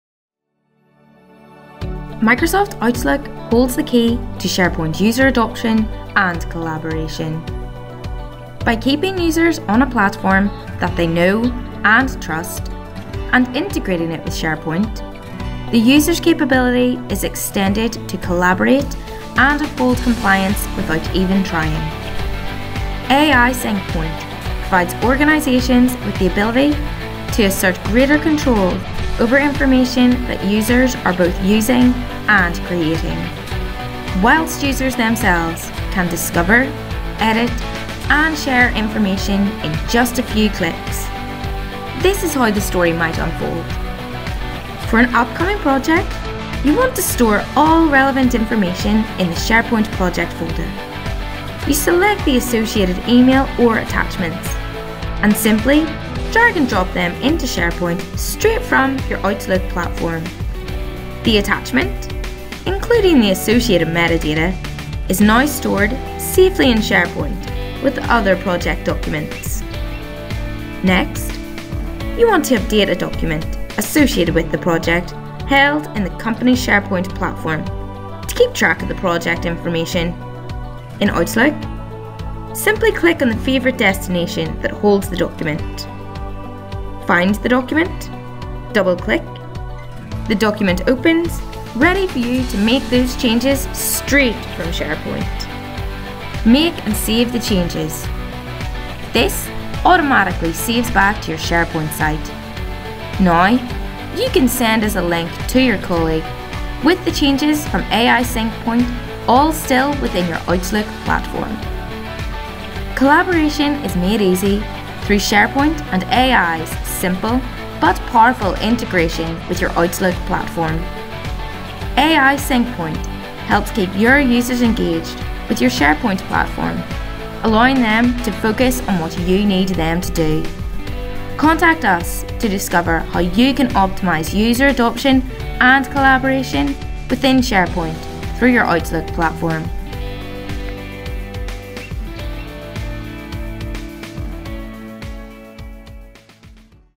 How to collaborate using Microsoft Outlook and SharePoint
User adoption of the SharePoint platform is key in order to maximise on the return of investment. This short video outlines how to encourage user adoption and collaboration of SharePoint through an integration with the Outlook platform. AI.SYNCPOINT - The simple, powerful SharePoint Outlook Plugin.